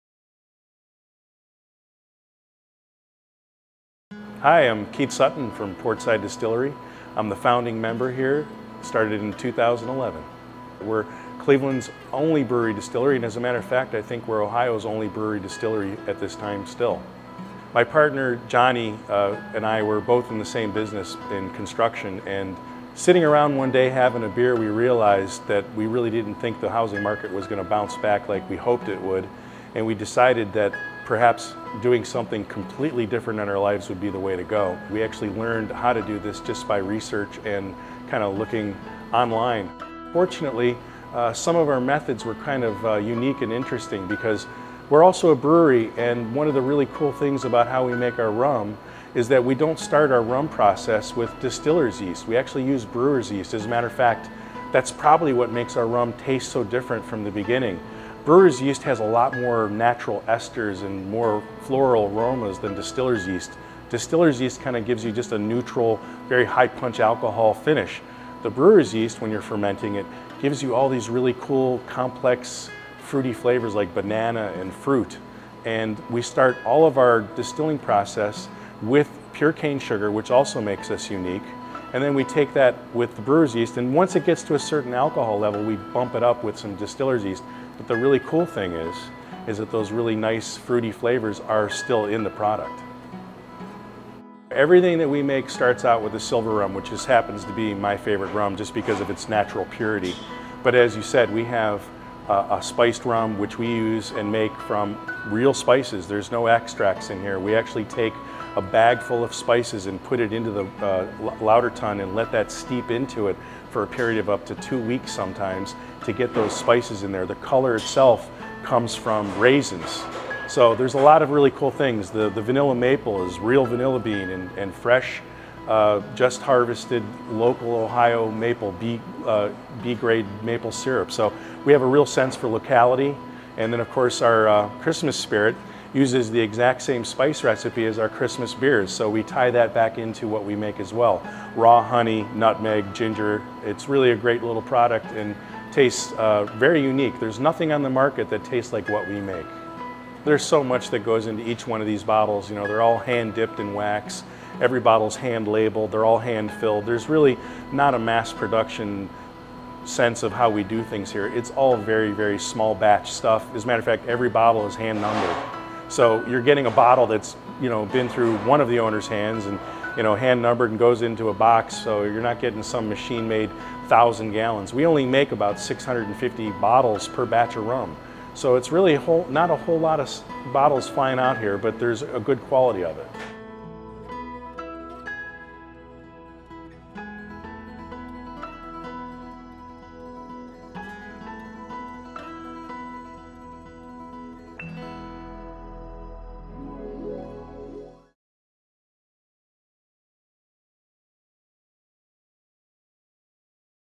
Portside Distillery
Portside Distillery is brewery/distillery that was started by two craftsmen in 2011.   With their ingenuity from construction and desire to make delicious rum, they craft small batch rum from pure cane sugar in a variety of flavors.   As Cleveland’s only brewery/ distillery, Portside has unique take on making rum and taste profiles to offer.